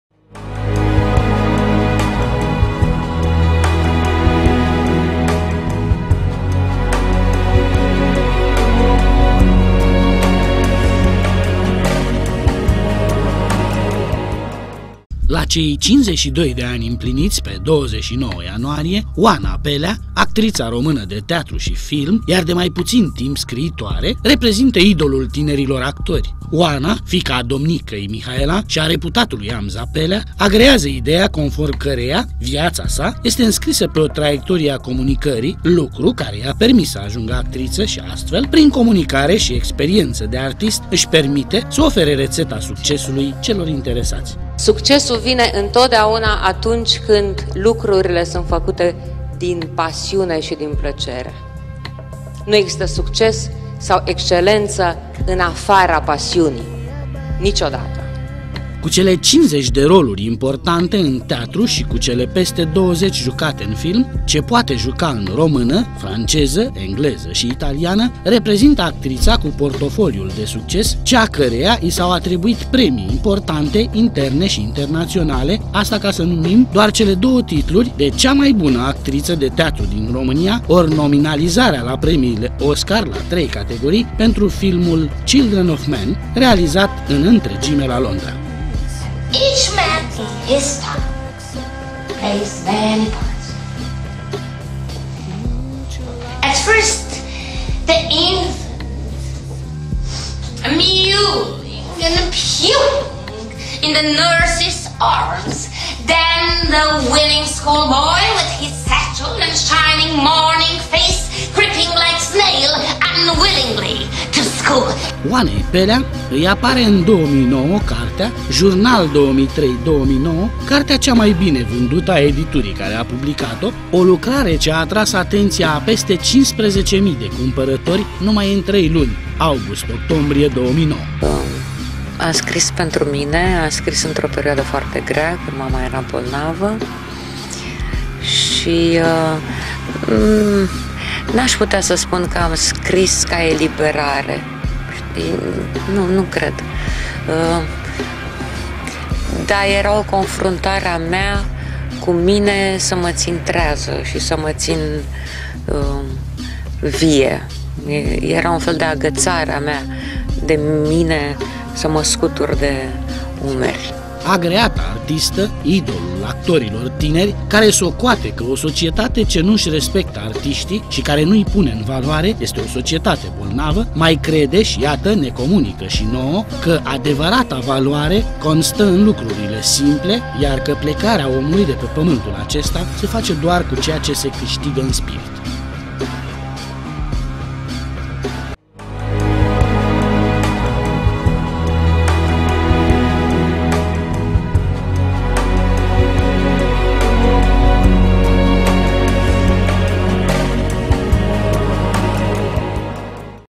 ACTRITA, SCRIITOARE SI IDOL
Idol al generației tinere de actori, Oana Pellea este actrița și scriitoarea născută pe 29 ianuarie 1962. Cu 50 de roluri importante în teatru și o filmografie numărând peste 20 de creații realizate în țară și străinătate, Oana a fost recompensată cu mai multe premii. În ceea ce privește succesul ori anumite secrete de viață, actrița-scriitoare are anumite rețete.